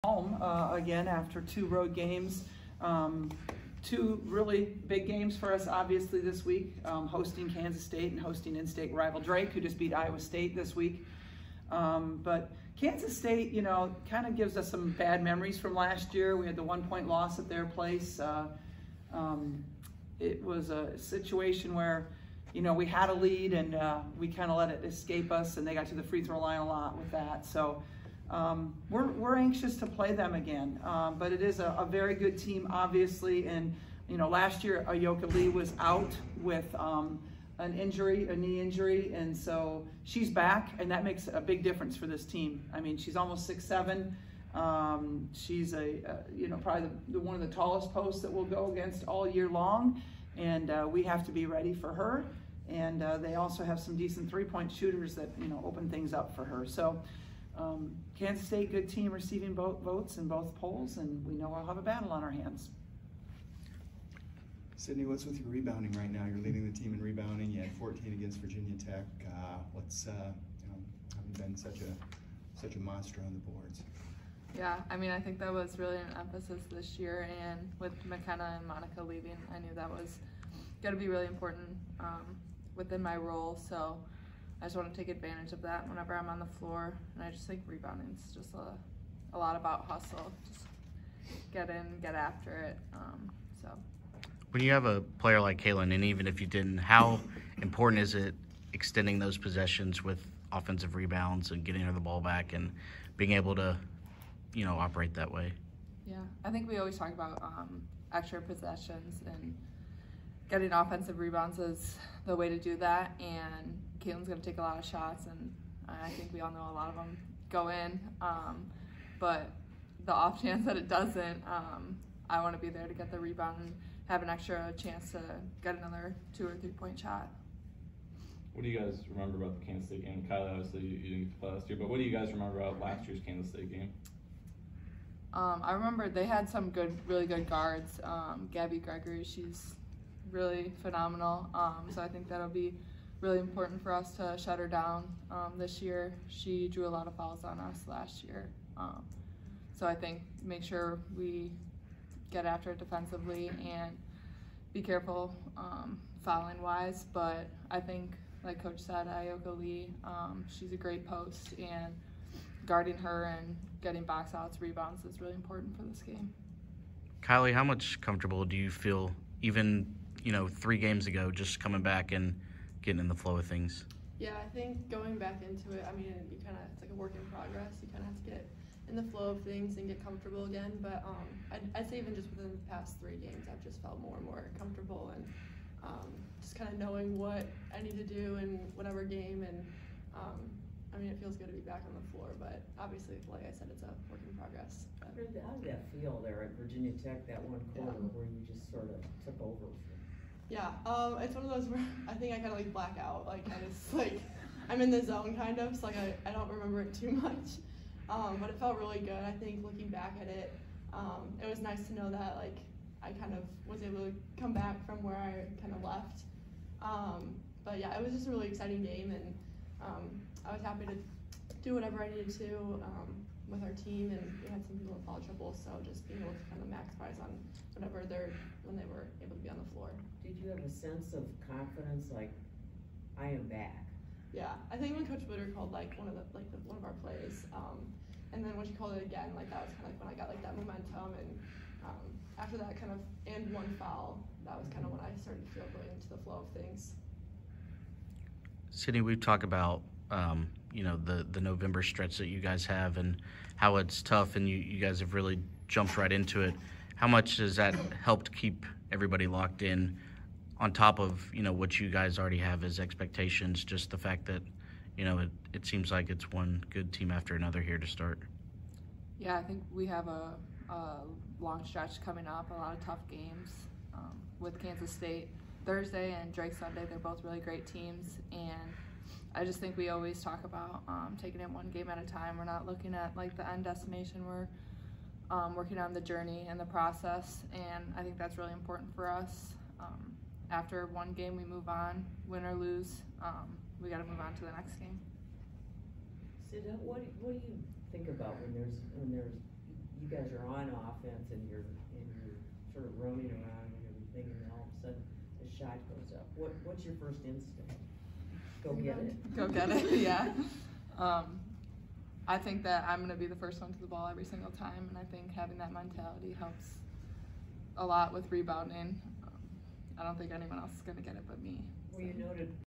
Sydney Affolter, Kylie Feuerbach preview Iowa women's basketball vs. Kansas State
Hear from Iowa women's basketball guards Sydney Affolter and Kylie Feuerbach ahead of the Hawkeyes' matchup vs. Kansas State.